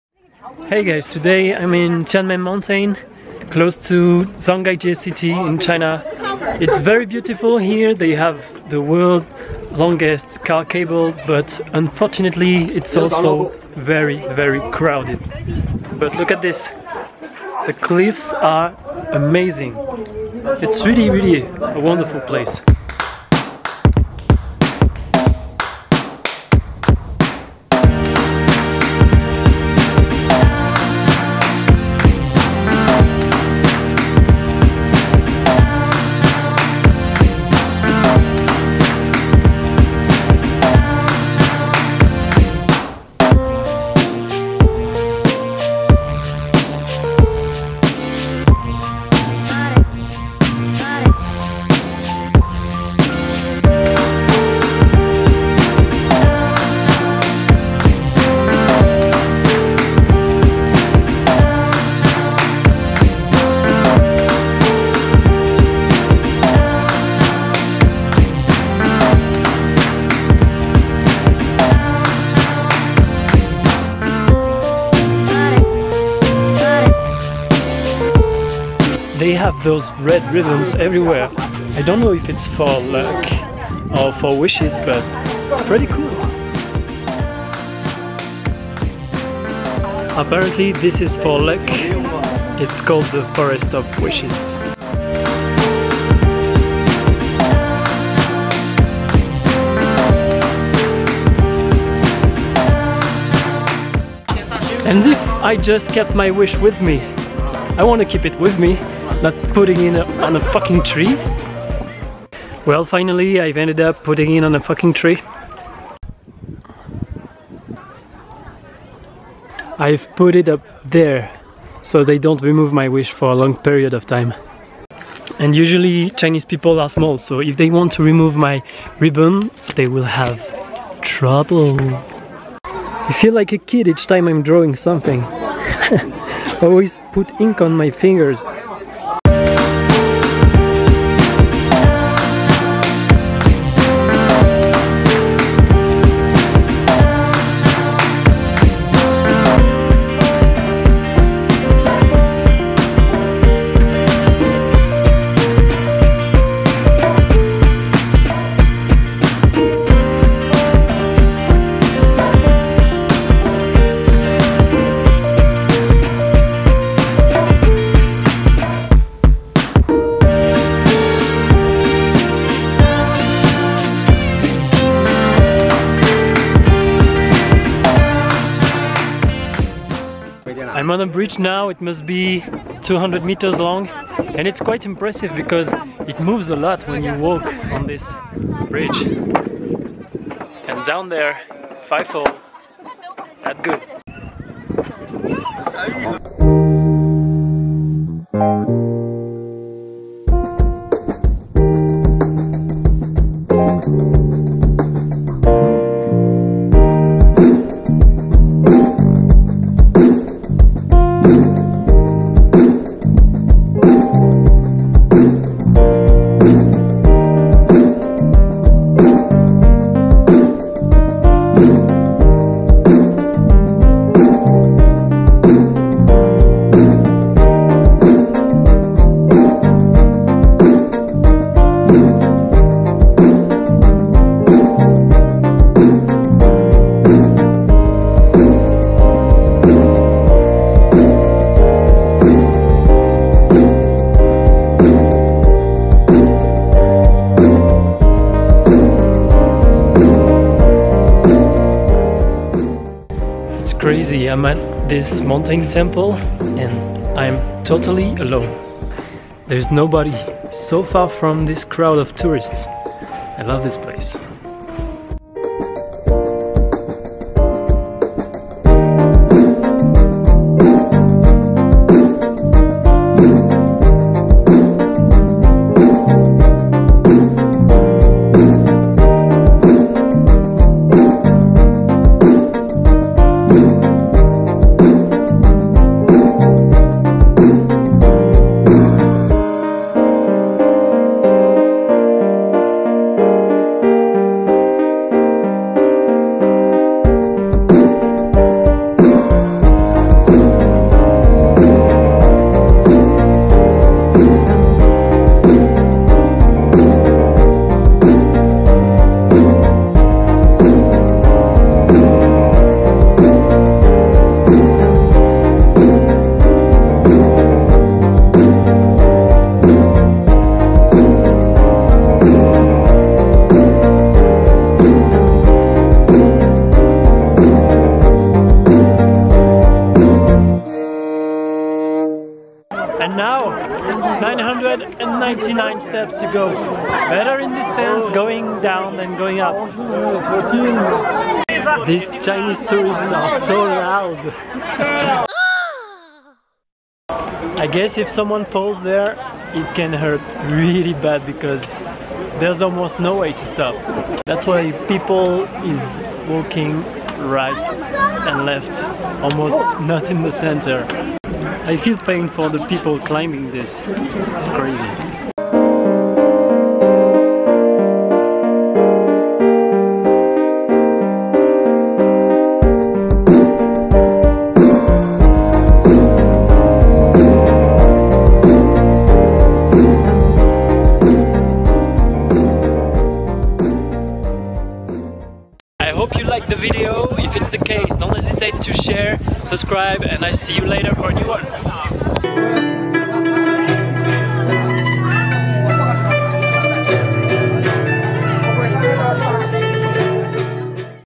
SKYWALK AND CLIFF HANGING WALKWAYS ! TIANMEN, CHINA
Beautiful and unique ! Cliff-hanging walkways, glass skywalk, and the longest cable car ride in the world.

Shot @ Tianmen mountain, China
You can access this mountain from the city of Zhangjiajie

👻 Add me on Snapchat! - or by username : MENAMCK

Particularities of Tianmen mountain :
- It has a beautiful road with an incredible snake/dragon shape
- It has a wonderful natural arch, and a the stairway leading to it, called "Stairway to Heaven's Door". It's also a famous place for wingsuit, some wingsuit flyers glided through it.
- Cliffhanging walkways, with just nothing below ! An impressive height, and breathtaking views.
- The longest passenger cableway of high mountains in the world, with 98 cars and a total length of more than 7km / 24 000 ft, an ascent of more than 1km with an unusual highest gradient of 37 degrees.

Mon matériel / my gear :
Canon Powershot G7X
Trépied Joby Gorillapod Hybrid
Trépied Joby GorillaPod Mini Magnétique